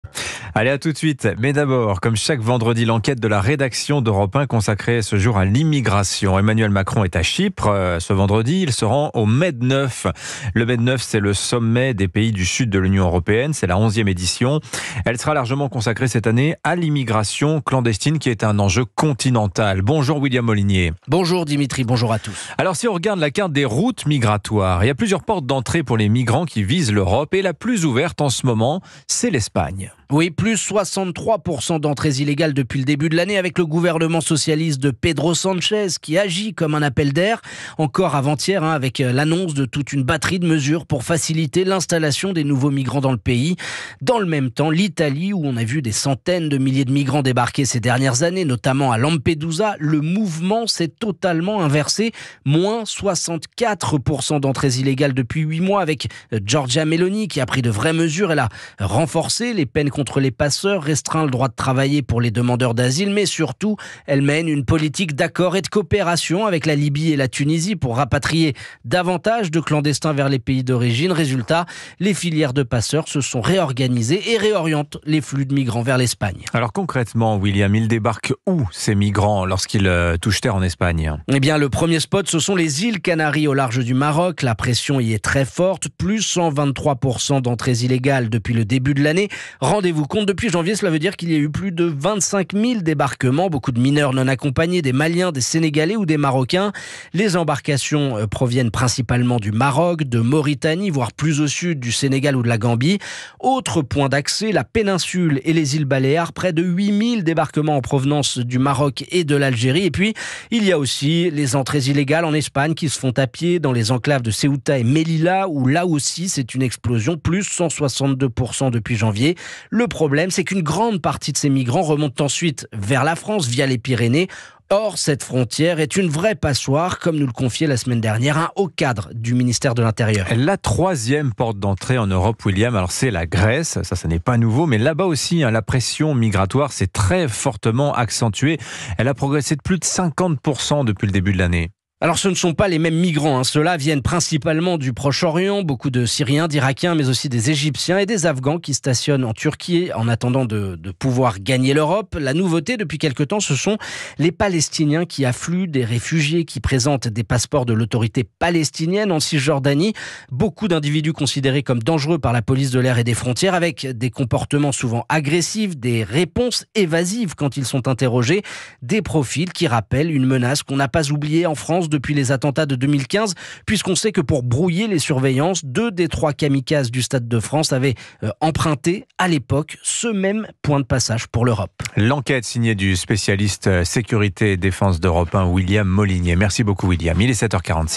Immigration clandestine : la nouvelle carte de l’Europe
Chaque vendredi, zoom sur un sujet qui fait l'actualité à l’étranger. Aujourd'hui, William Molinié s'intéresse à l'immigration clandestine en Europe, l'Espagne et la Grèce sont confrontées à une explosion des d’entrées illégales.